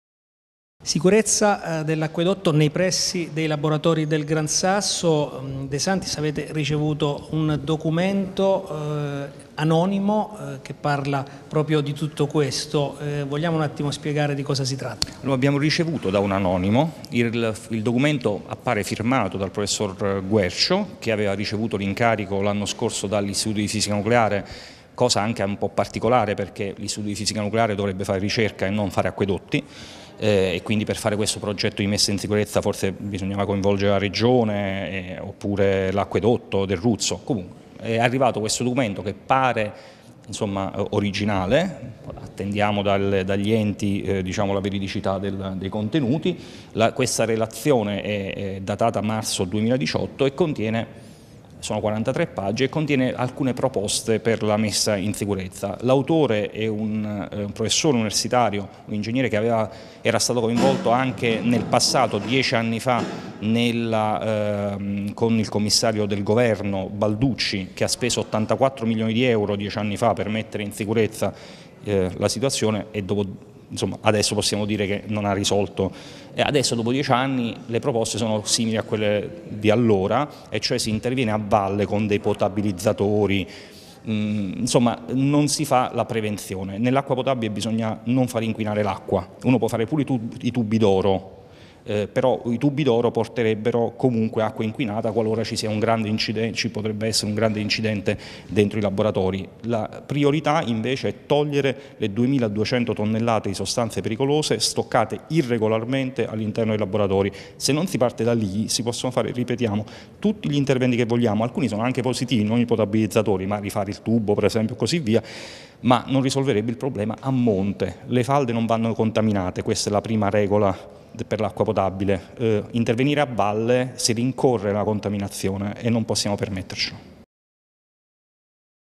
Gran Sasso Leaks documento anonimo sulla messa in sicurezza
Il Forum abruzzese dell'Acqua ha ricevuto  un documento di 43 pagine che contiene alcune proposte per la messa in sicurezza dell'acquifero del Gran Sasso; il documento è stato recapitato alla Mobilitazione per l'Acqua del Gran Sasso e all'Osservatorio da un anonimo un paio di giorni fa.